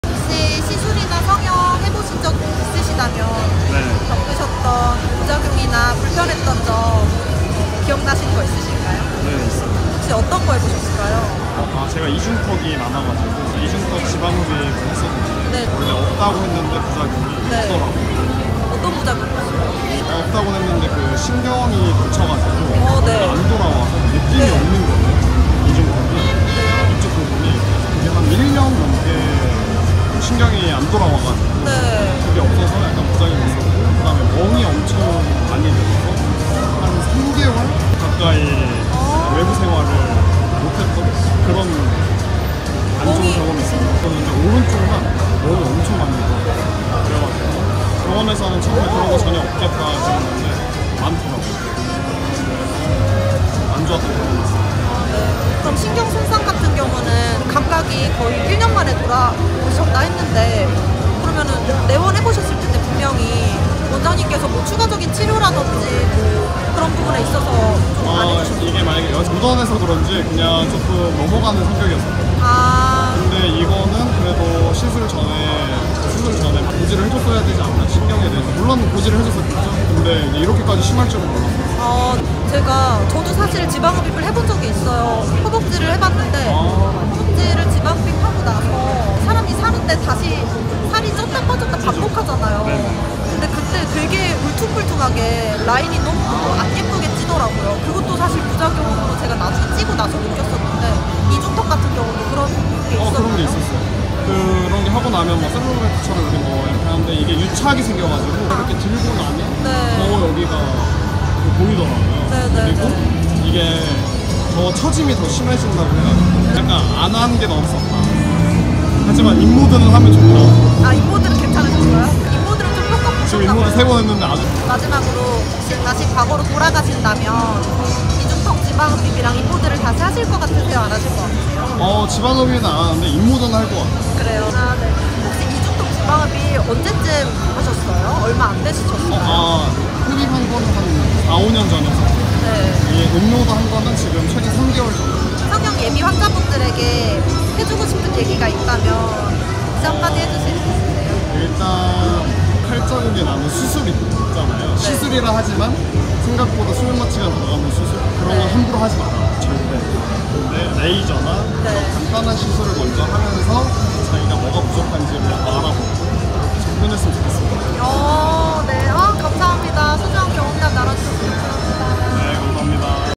"이정도로 심할지는 몰랐죠." (ft.이중턱 지흡 부작용)
솔직한 경험담 나누어주신 게스트 분께 다시 한번 감사드립니다.
성형을 고려중이신 많은 예비 환자분들에게 도움이 되었으리라 생각합니다.

►Info
- 👗 (의상대여)
오늘룩

(착용제품 등은 인스타에서 확인하실 수 있습니다)

직접 겪으신 소중한 부작용 경험담을 나누어주세요.☺️